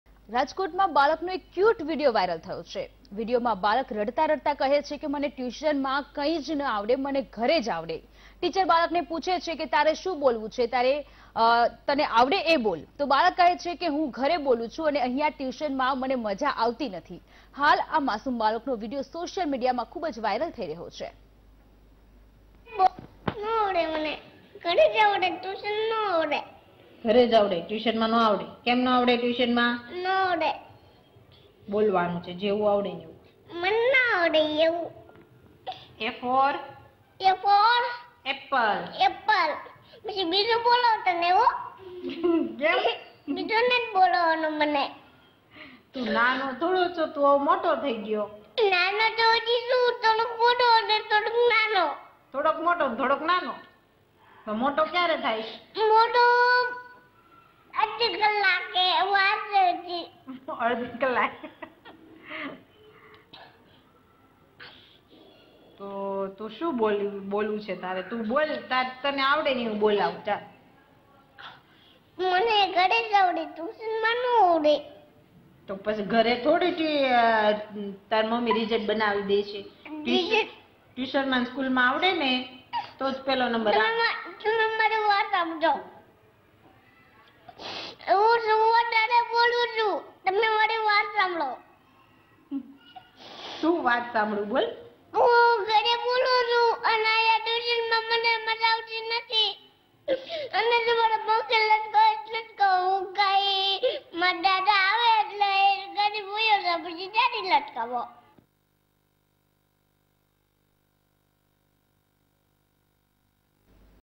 VIDEO : મને ઘરે જ આવડે ટ્યુશનમાં ન આવડે, રડતાં રડતાં કાલીઘેલી ભાષામાં બોલતા માસુમે જીત્યું દિલ
VIDEO :  મને ઘરે જ આવડે ટ્યુશનમાં ન આવડે, રડતાં રડતાં કાલીઘેલી ભાષામાં બોલતા માસુમે જીત્યું દિલ | VTV GUJARATI